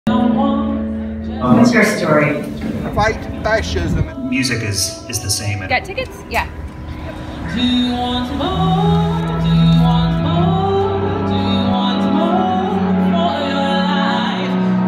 True/False Film Festival 2016 - Day 3
Day 3 - A short visual diary from attending one of the best documentary film festivals around.

Shot and edited on iPhone and originally published to Instagram.